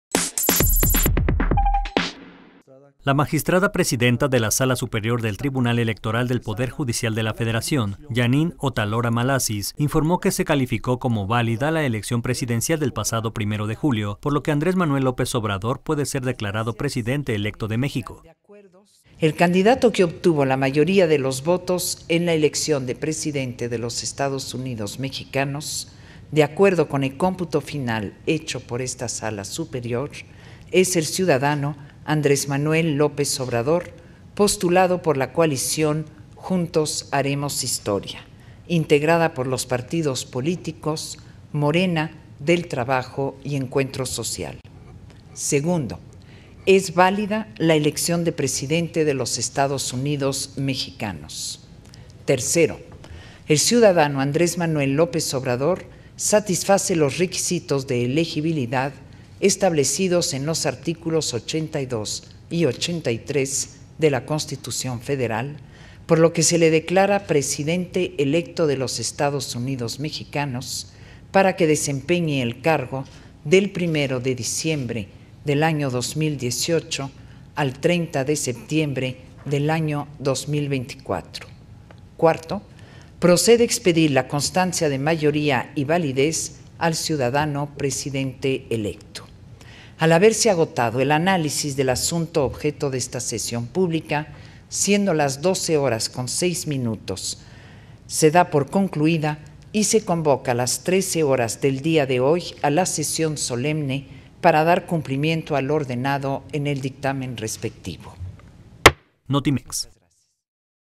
Válida la elección; López Obrador es presidente electo: Tribunal Electoral
La magistrada presidenta de la Sala Superior del Tribunal Electoral del Poder Judicial de la Federación (TEPJF), Janine Otálora Malassis, informó que se calificó como válida la elección presidencial del pasado 1 de julio, por lo que Andrés Manuel López Obrador puede ser declarado presidente electo de México.